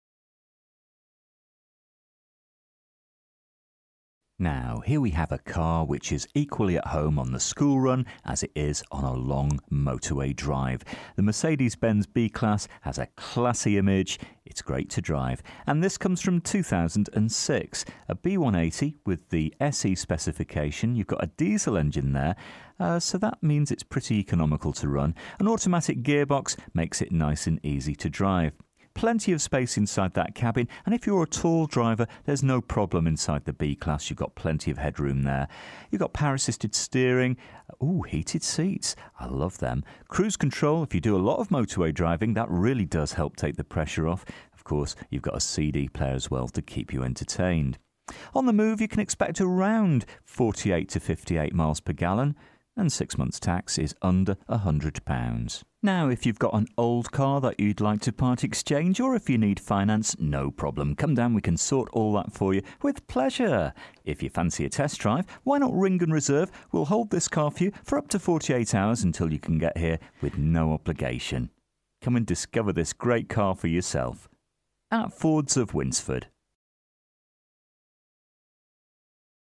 USED MERCEDES-BENZ B CLASS DIESEL HATCHBACK (2006) B180 CDI SE 5DR TIP AUTO - AO56GGU
MERCEDES-BENZ B CLASS DIESEL HATCHBACK (2006)  B180 CDI SE 5DR TIP AUTO

DETAILS:
First registered: 29/09/2006
Current mileage: 54,210
Registration: AO56GGU
Colour: GREY

FEATURES:
all round electric windows, remote central locking, cloth interior, all round airbags, air conditioning, pas, abs, alloy wheels, metallic paint, cd, heated seats, traction control, parking control, load space cover present, hand book present, cruise control